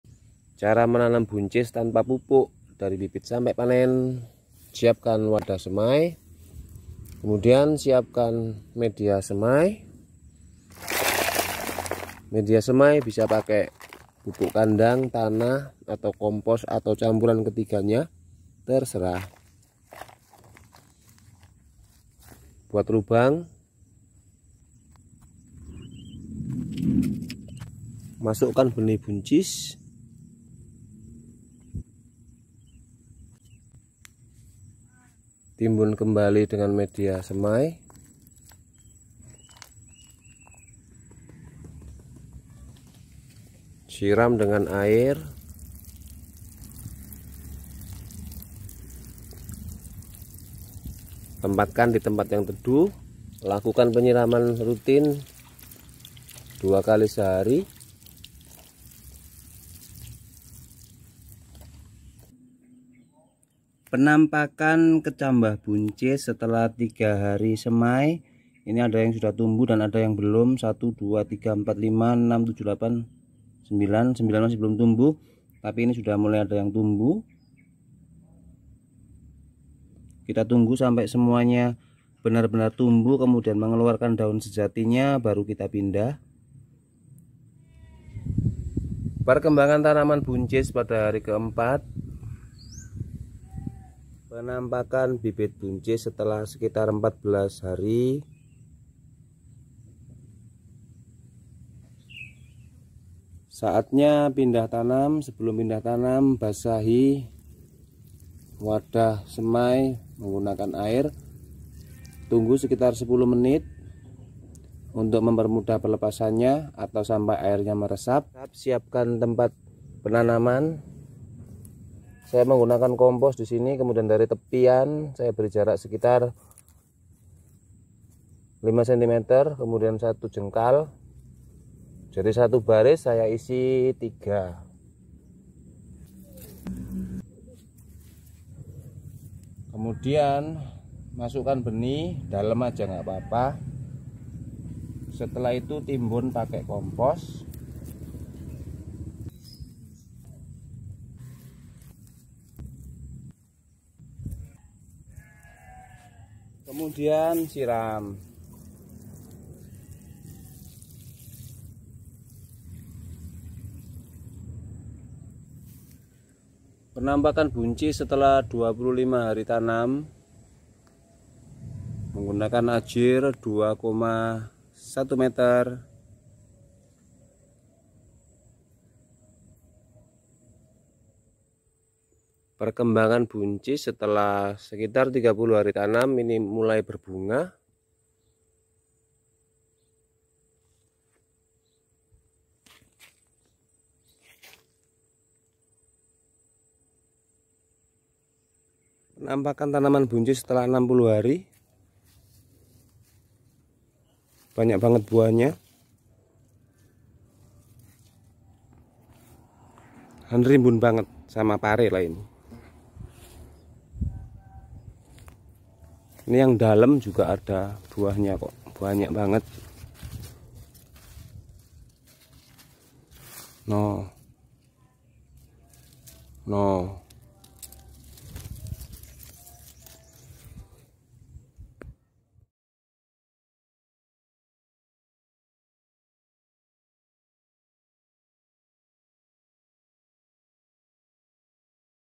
Cara Menanam Buncis dari Biji Sampai Panen
Cara menanam Buncis organik menngunakan kompos tanpa pupuk tanpa pestisida dari semai sampai panen